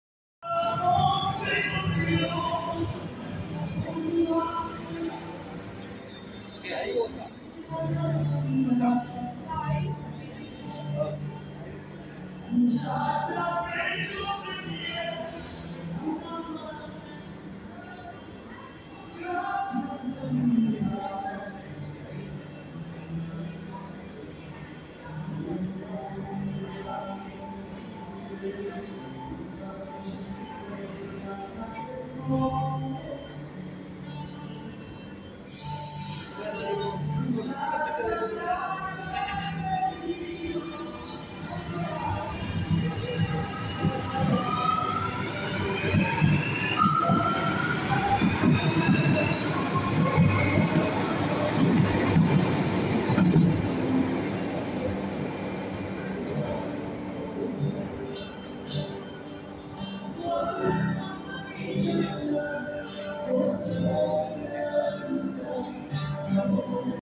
Subte Línea H: Formación Alstom 312 Equipo (L) En La Estación Santa Fe - Carlos Jáuregui.
En este vídeo vemos a la Formación Alstom Metrópolis 312 de la Línea H de Subtes de Emova ingresando y saliendo de la estación Santa Fe - Carlos Jáuregui rumbo a Hospitales.
Composición: Rca312, Ma312, Mb312/Mc312, Md312 y Rcb312.
Linea Premetro los trenes no se detienen en la estación Ana Maria Janer por obras de mejora.

Filmado el día Viernes 29/07/2022 a las 15:50 hs.